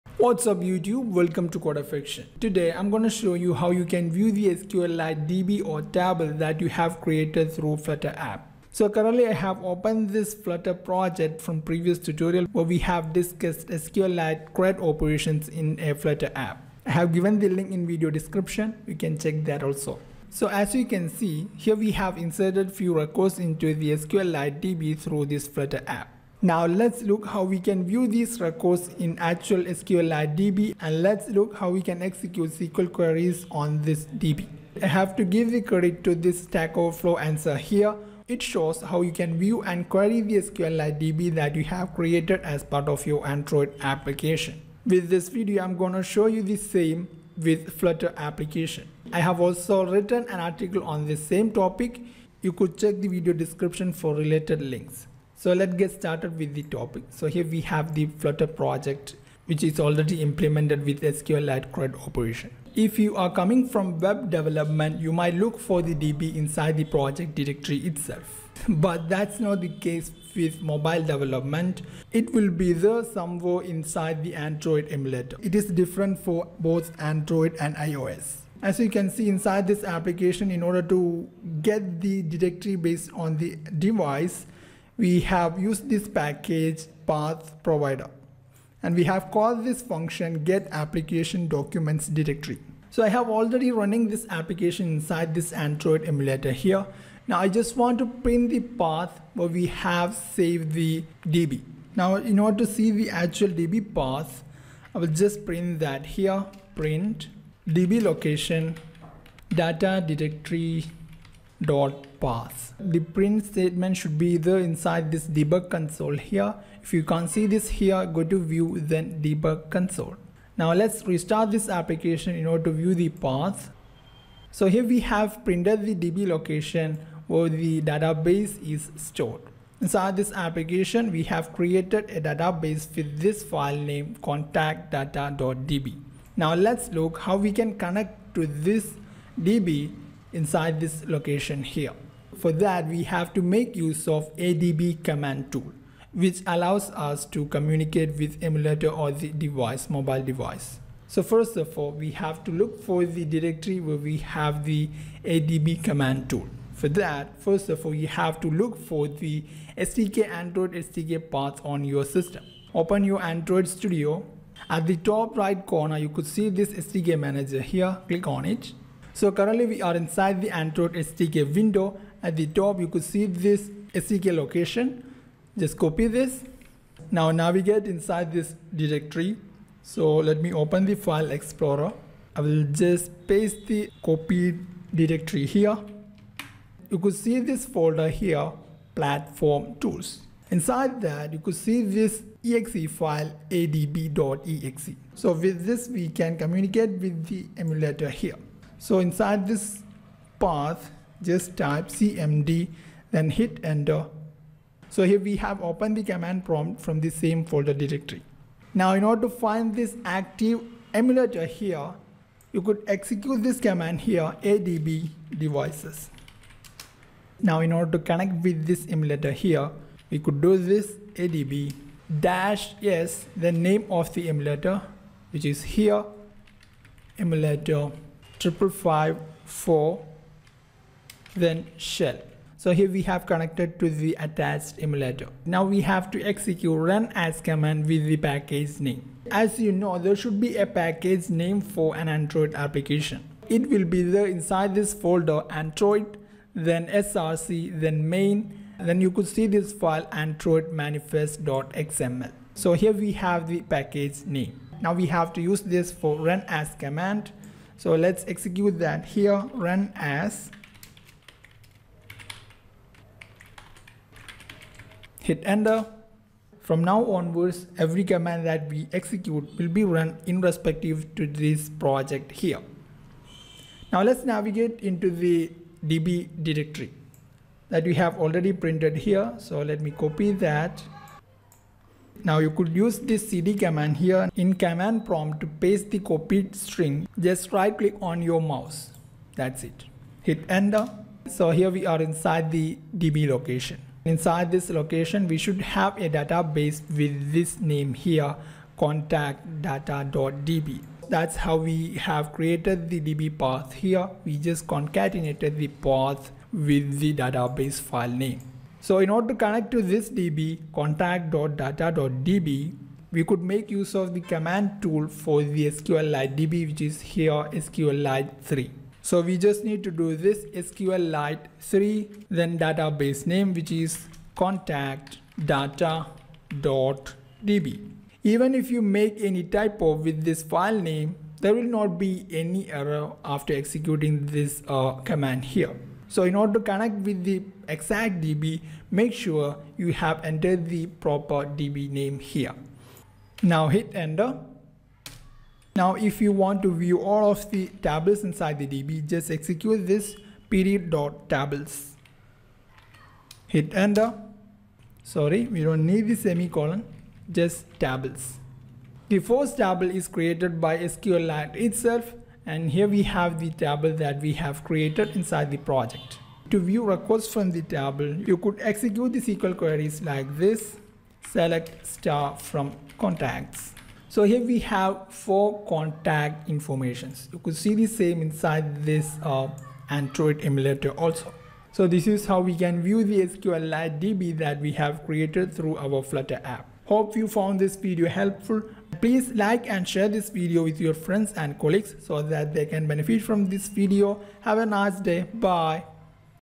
View SQLite Database in Flutter App
View SQLite databases in Flutter App

How to view and query the SQLite Database in Flutter application with the help of ADB command tool and SQLite3.

: SQLite CRUD Operation in Flutter